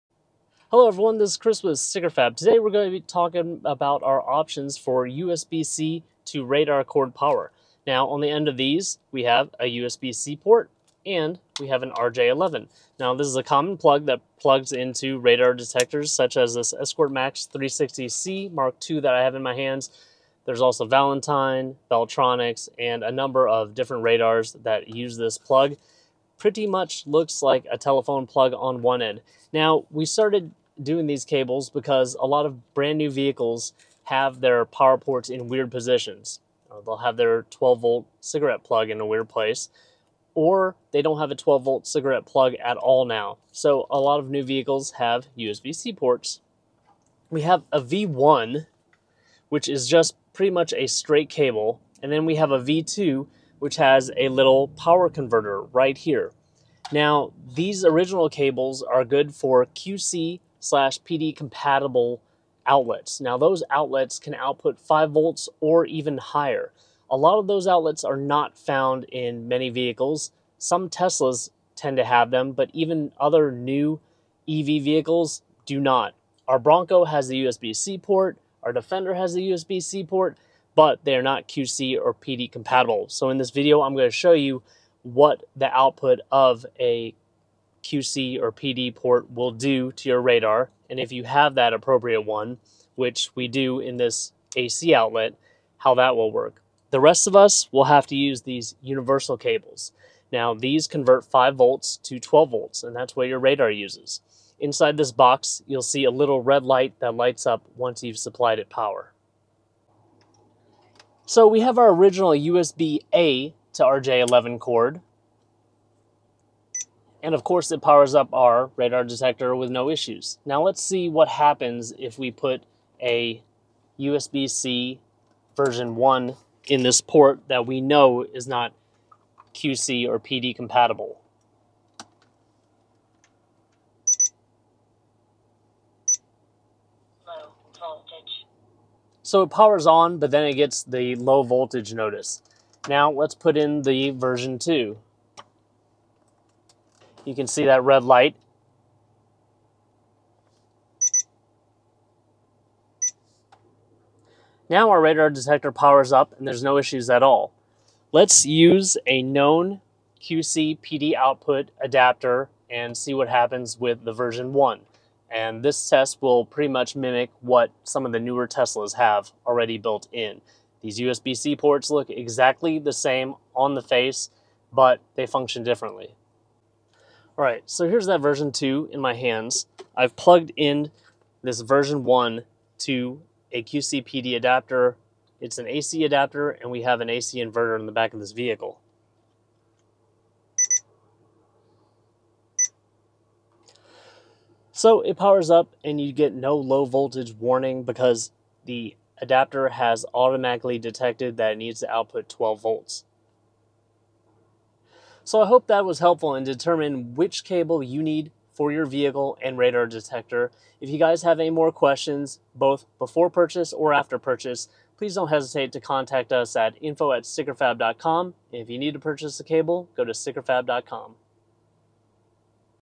USB-C to RJ-11 Adaptor for Valentine 1, Escort, Uniden, Beltronics
Here's a solution that prevents messy wires that we use ourselves!  It is a universal part,

This takes a USB-C Plug (PD/QC Compatible or standard 5V) and gives you an RJ11 12V jack which is compatible with many radar detectors from popular manufacturers like Escort, Valentine, and Uniden.

Please check your radar manufacturer for specifications.

1 year warranty.  Be careful not to place tension on the wire.  This can break the casing and bend the connection between the PCB, port, and internal wires.